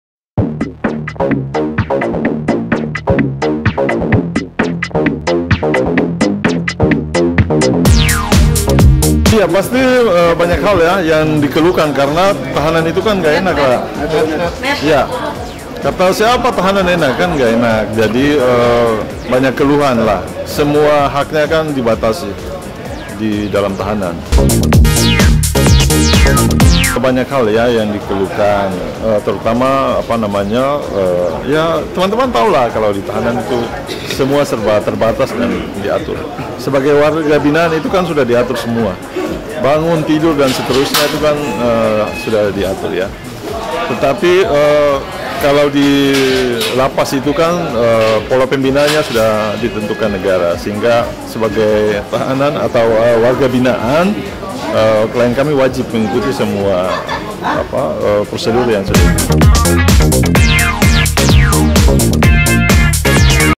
Pengacara Ungkap Keluhan Jennifer Dunn Selama di Tahanan
Akibat tersangkut kasus penyalahgunaan narkoba, Jennifer Dunn ditahan di Rutan Pondok Bambu, Jakarta Timur, sejak beberapa waktu lalu.

Berada dalam sel tahanan, apa saja kegiatan yang dilakukan Jennifer Dunn? Lalu apa saja yang dikeluhkan Jennifer Dunn?

Ditemui seusai sidang perdana Jennifer Dunn di Pengadilan Negeri Jakarta Selatan, Pieter Ell selaku kuasa hukum Jennifer Dunn, menjelaskan pengalaman Jennifer Dunn selama di tahanan.